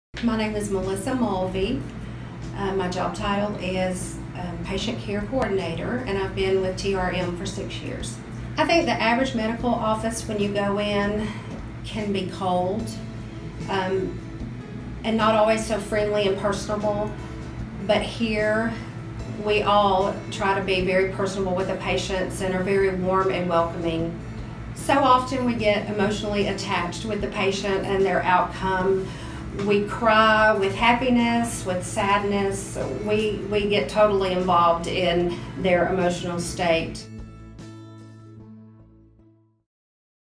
Meet Melissa Mulvey
Melissa Mulvey is a patient care coordinator at Tennessee Reproductive Medicine in Chattanooga, TN.